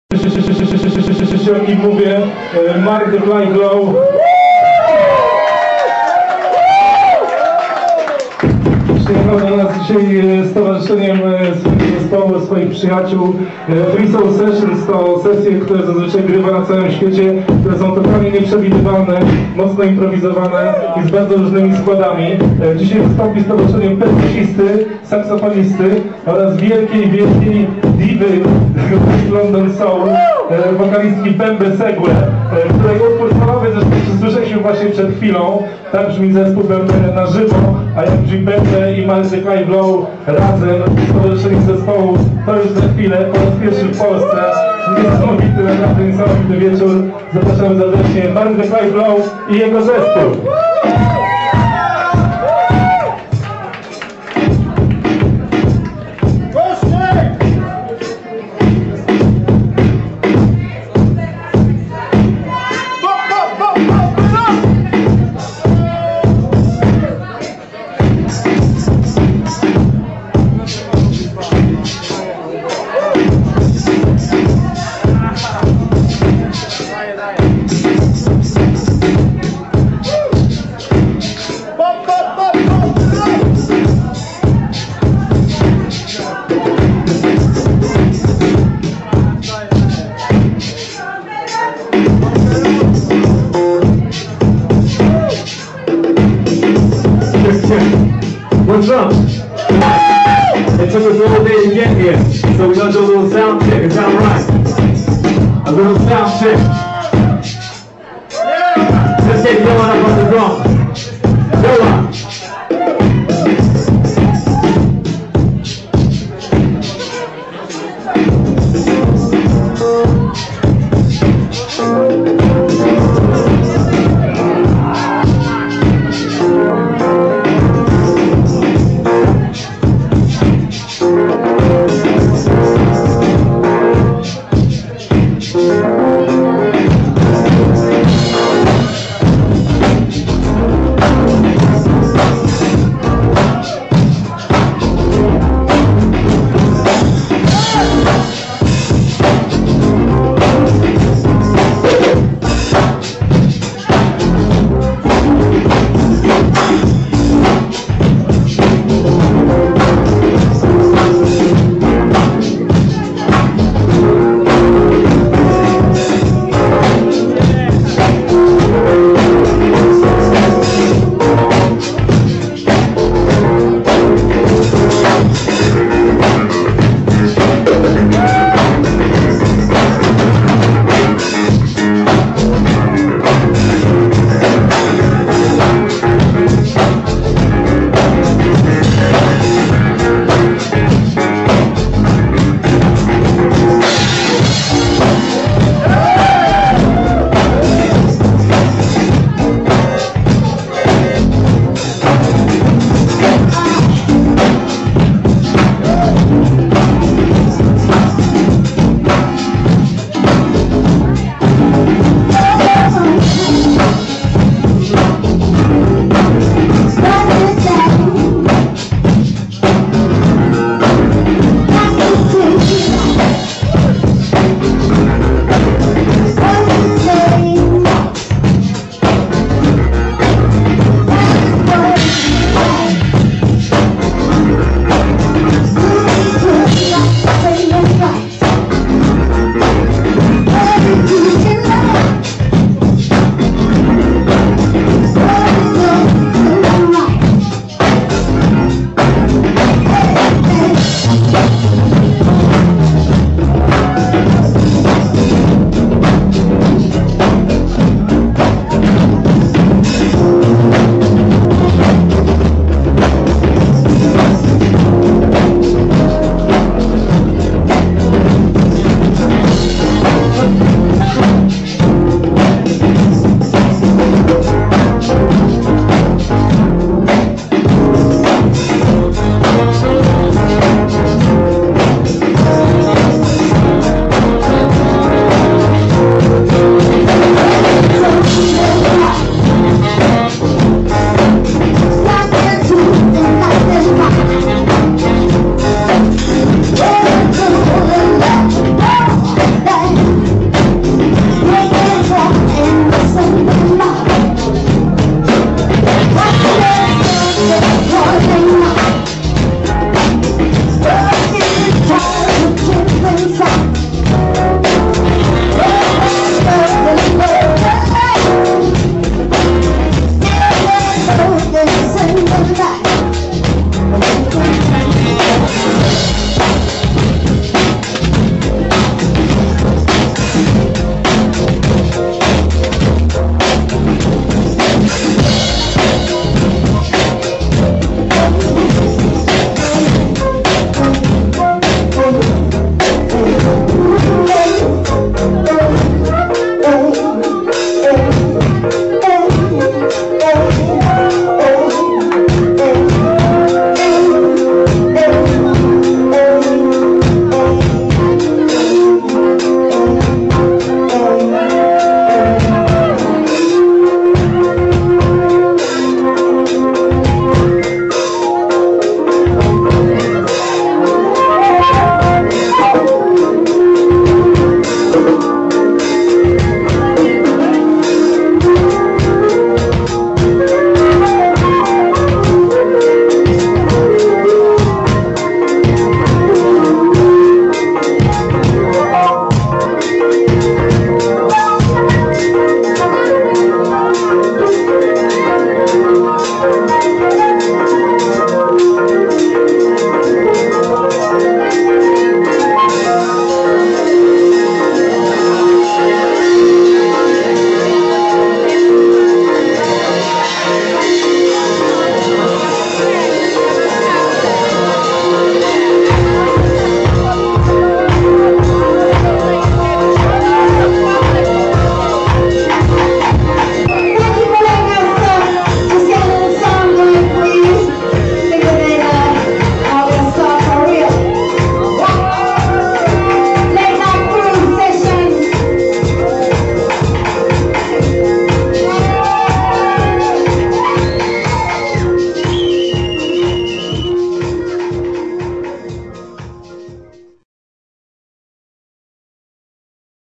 Mark declive lowe - freesoul session @ "Vavavoom" in Zakopane - Poland (april 2006)
mark declive lowe - keys,mpc etc...

bembe segue - vocals

yoann julliard - drums

ben hadwen - sax